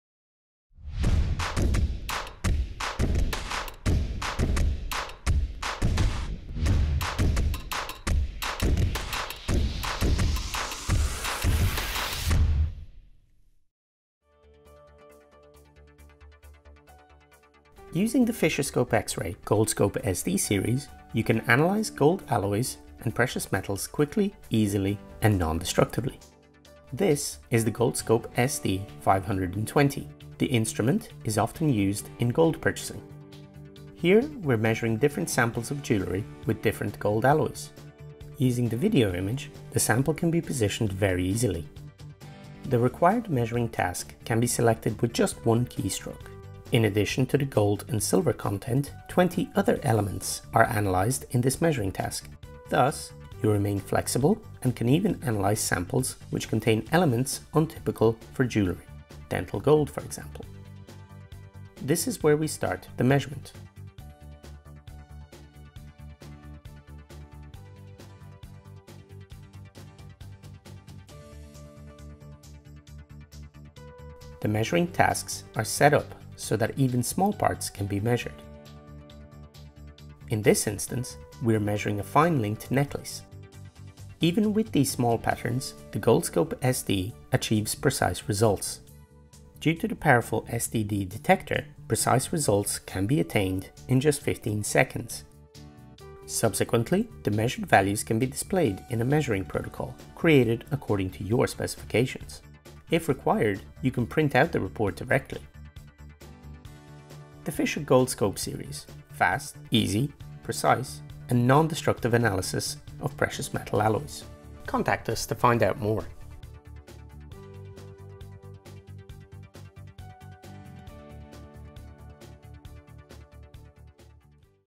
Material Analysis of Gold & Precious Metals – Fast, Precise, Nondestructive | GOLDSCOPE SD | Fischer
Benefit from our expertise in the authenticity testing of gold, jewellery, watches and coins!
With the GOLDSCOPE SD® series, you can analyse gold alloys and precious metals quickly, easily, precisely and non-destructively.
Whether in pawn shops, testing laboratories, refineries, gold purchasing or assaying offices – the features of the GOLDSCOPE SD® models assist you optimally and make your daily work as easy as possible. The required measuring task can be selected with just one keystroke. The most important measuring tasks for gold and precious metals are already pre-programmed. Due to the powerful SDD detector, you attain precise results within 15 seconds.